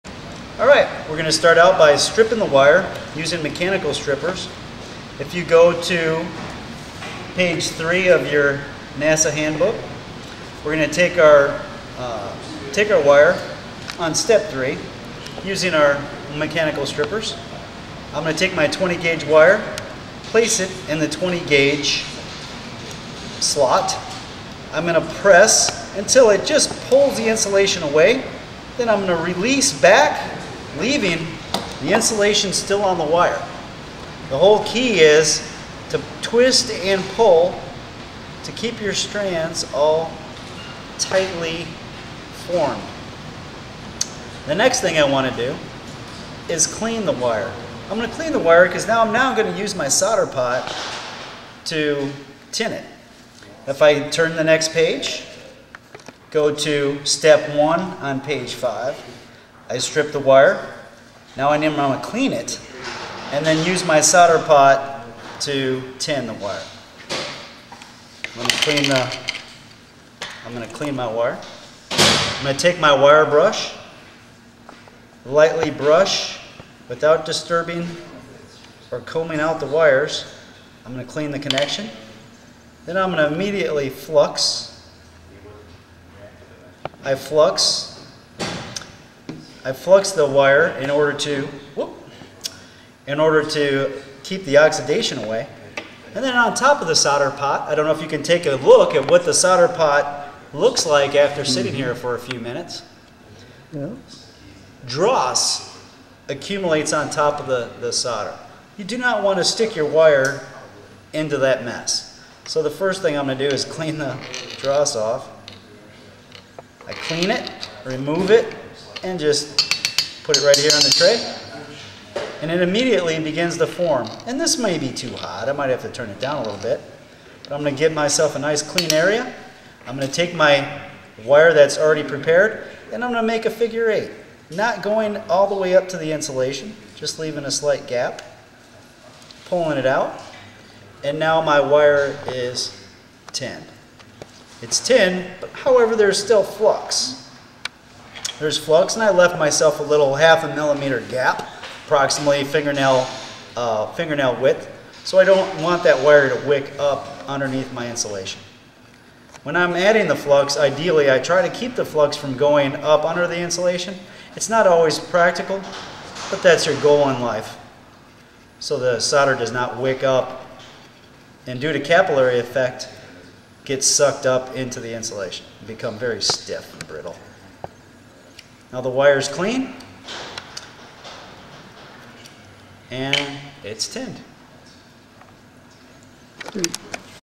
*EXPERT LEVEL* Soldering Tutorial #2 TTC Avionics
NASA Certified Instructor Mike Bunting demonstrates how to properly tin a wire using a solder pot.